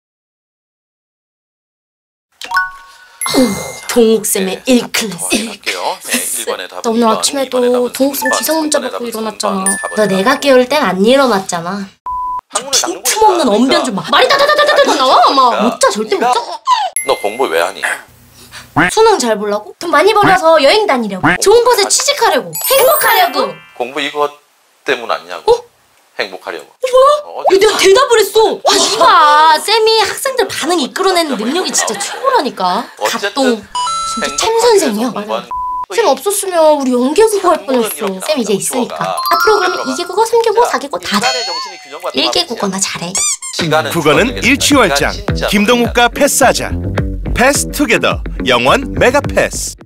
[메가스터디] 현역 고딩들 국어 김동욱쌤 참교육에 홀려 반응한 썰
지금 대답한 사람 손~! 너도? 야 나두!⚆o⚆

나... 드디어 1개 국어 하게 되는 건가!

국어 성적 레벨업하고 싶다면
국어는 일취월장, 김동욱과 패스하자!